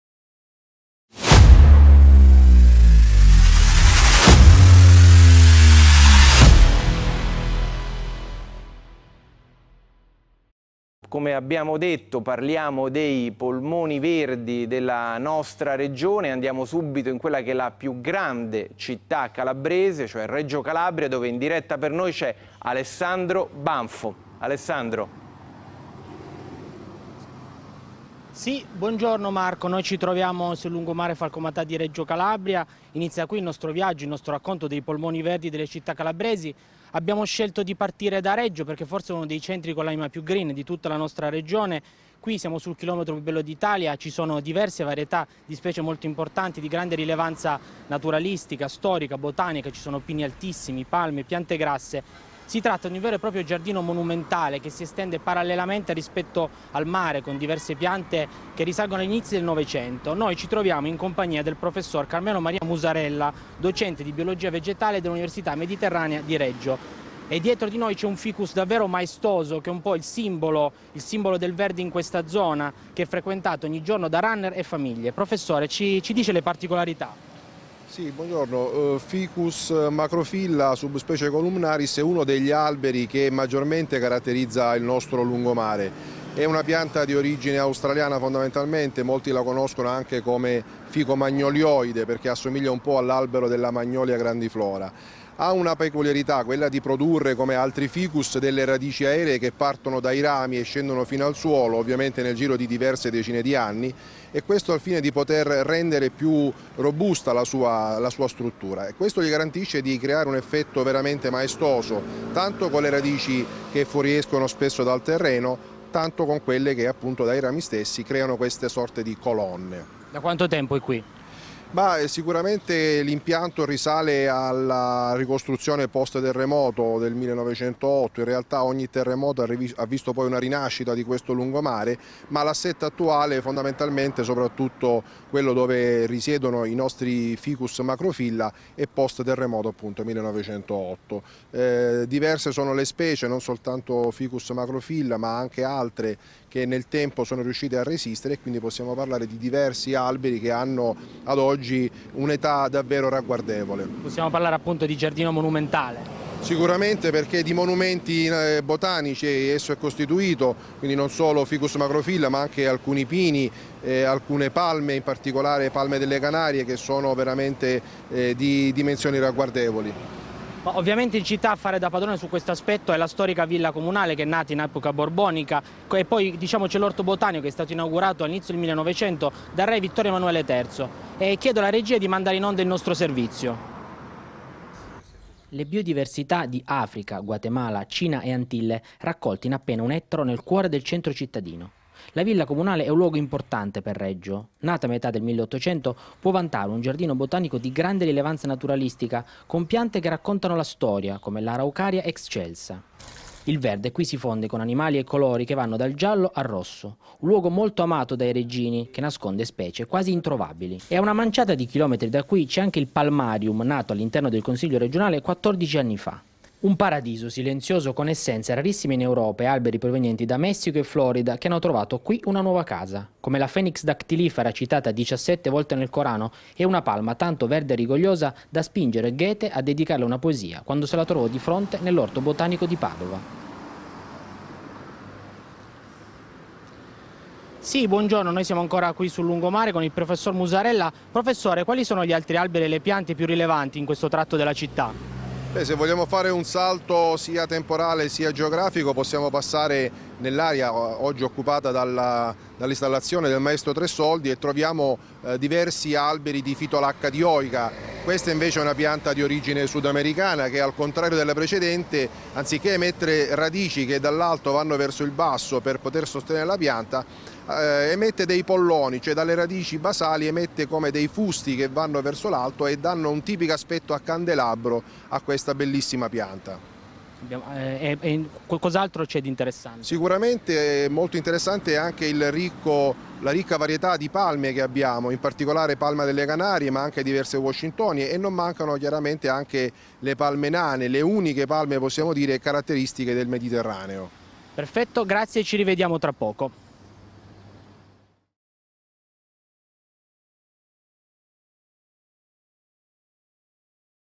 Un tesoro chiamato natura
A Reggio piante secolari arricchiscono il lungomare e la Villa comunale.

Nella città dello Stretto coesistono in pieno centro un giardino monumentale sul lungomare, una Villa comunale antichissima e un Palmarium.

Intervista a Carmelo Maria Musarella, docente di Biologia vegetale dell’università Mediterranea